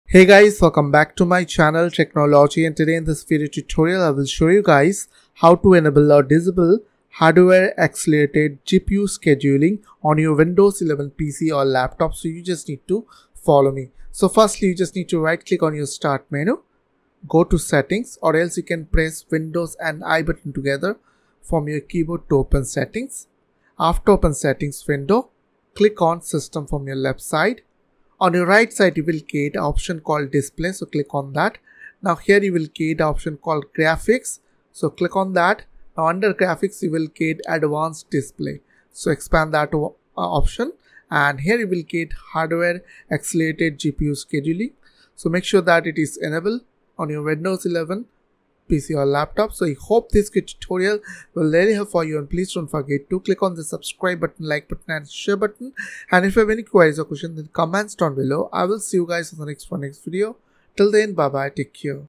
How To Turn on or off Hardware Accelerated GPU Scheduling on Windows 11
Here in this video tutorial, I will show you guys how to turn on or turn off "Hardware Accelerated GPU Scheduling" on Windows 11.